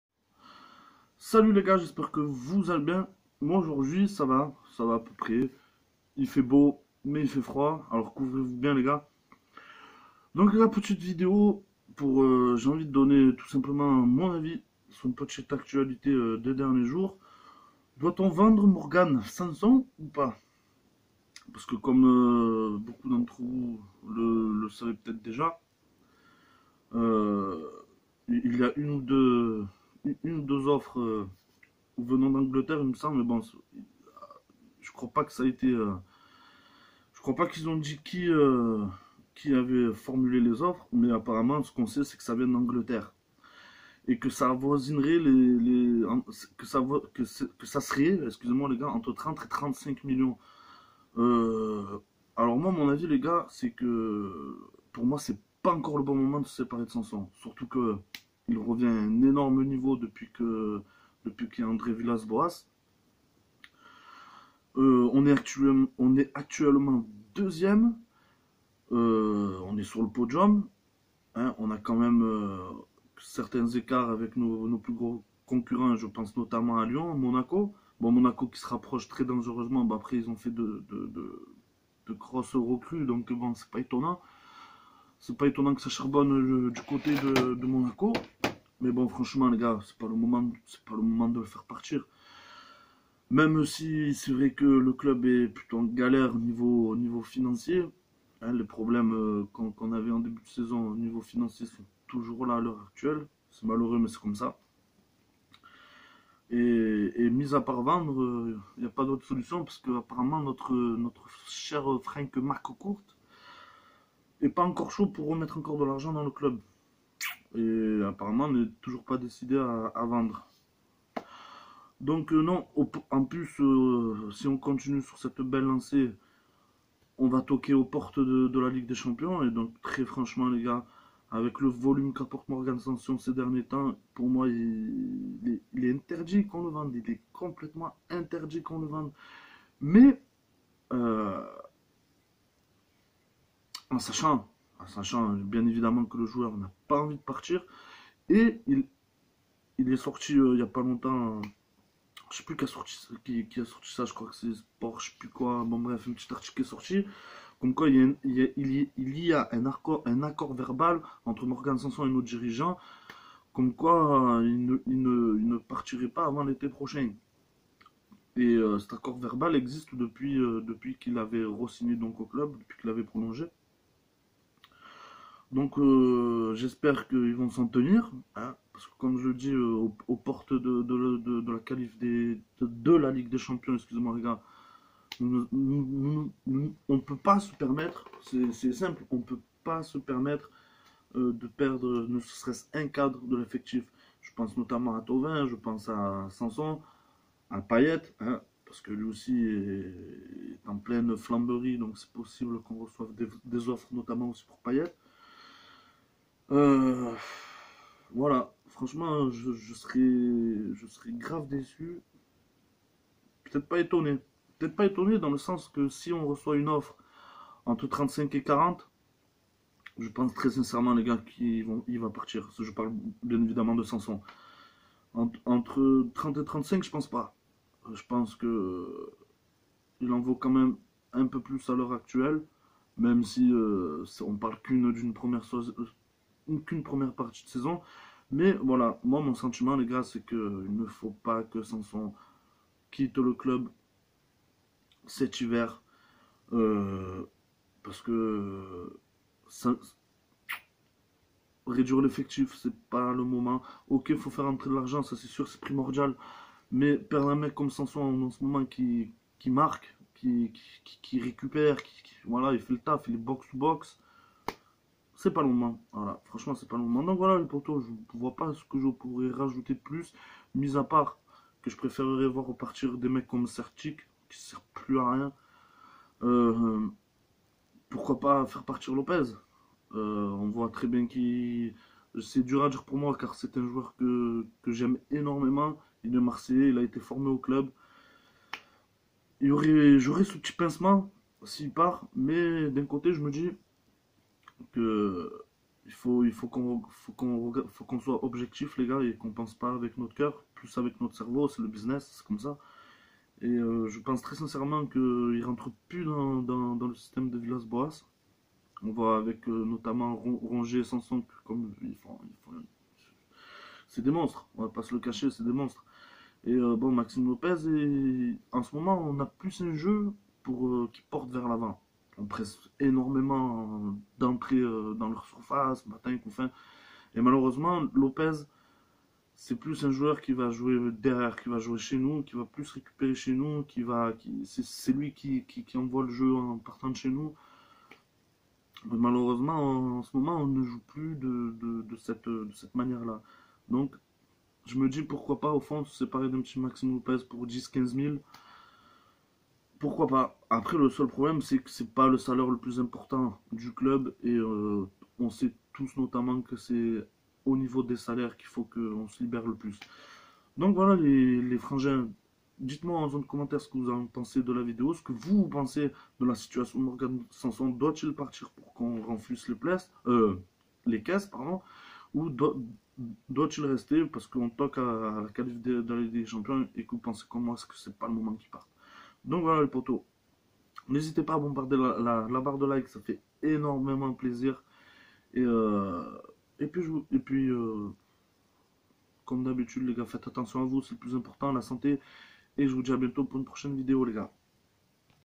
MORGAN SANSON DOIT-IL PARTIR ?
Salut tout le monde une petite vidéo pour vous donner mon avis sur le départ ou non de margane sanson